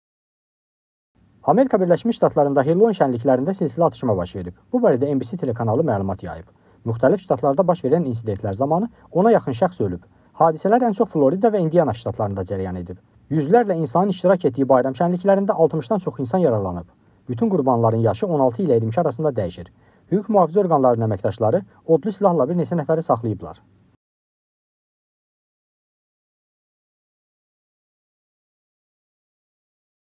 ABŞ-da atışma, ölənlər var – APA TV
ABŞ-də Hellouin şənliklərində silsilə atışma baş verib. Bu barədə NBC telekanalı məlumat ayayıb. Müxtəlif şatatlarda baş verən insidentlər zamanı 10-a yaxın şəxs ölüb. Hadisələr ən çox Florida və İndiana ştatlarında cərəyan edib. Yüzlərlə insanın iştirak etdiyi bayram şənliklərində 60-dan çox insan yaralanıb. Bütün qurbanların yaşı 16 ilə 22 arasında dəyişir. Hüquq-mühafizə orqanlarının əməkdaşları odlu silahla bir neçə nəfəri saxlayıblar.